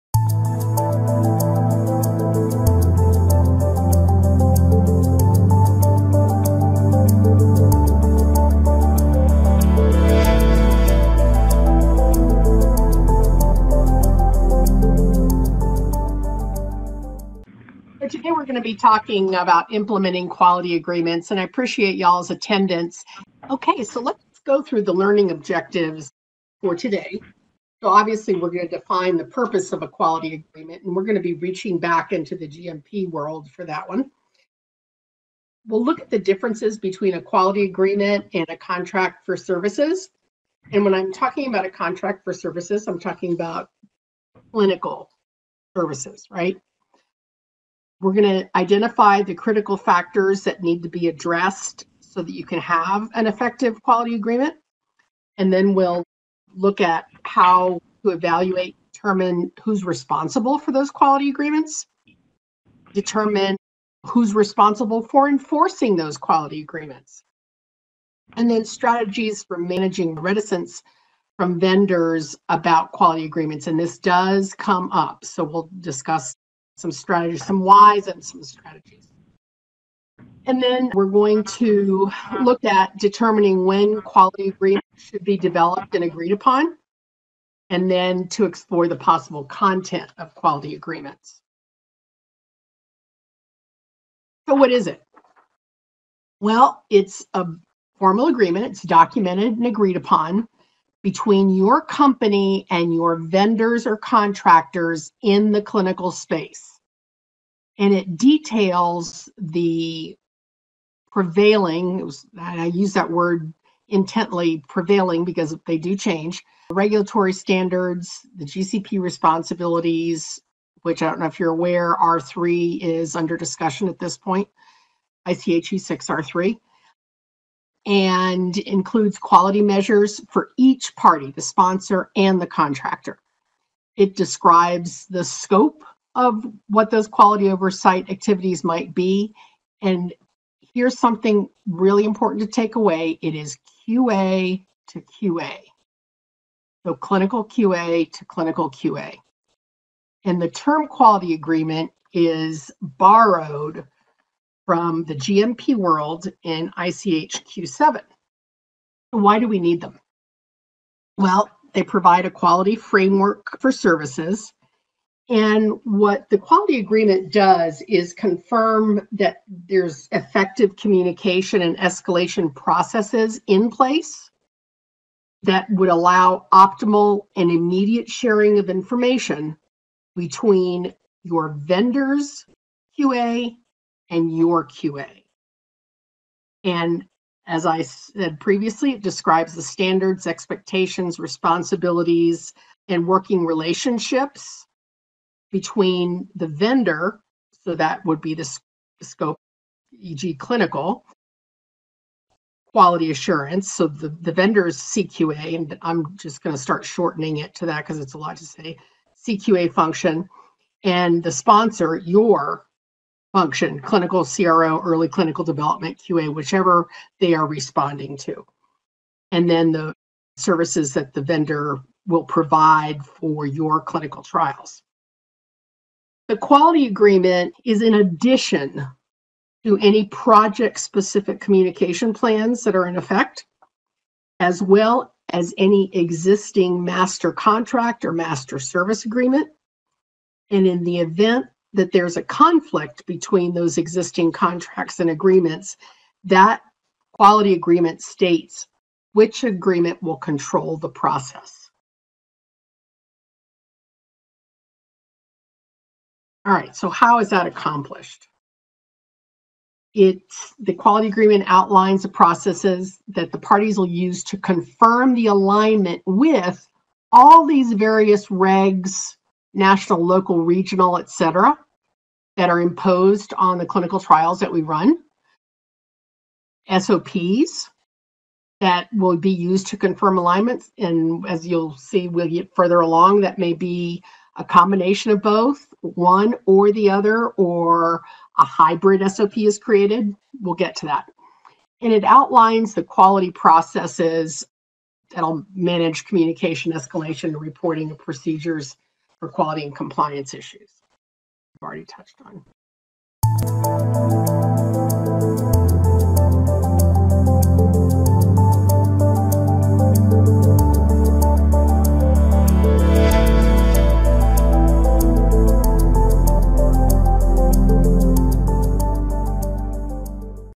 Implementing Quality Agreements
Quality Agreements are a mainstay of the Good Manufacturing Practice (GMP) sphere as defined in ICH Q7 Section 16.12, and in Good Laboratory Practice (GLP) as inferred in 21 CFR Part 58.1 and 58.35. They have become more common in the Good Clinical Practice (GCP) arena of clinical trial operations as risk-based study management becomes firmly established. Quality Agreements delineate the quality expectations of both the sponsor and outsourced provider and are structured in a shared agreement. Web seminar participants will learn the purpose of, factors that are considered in, and explore possible content of Quality Agreements.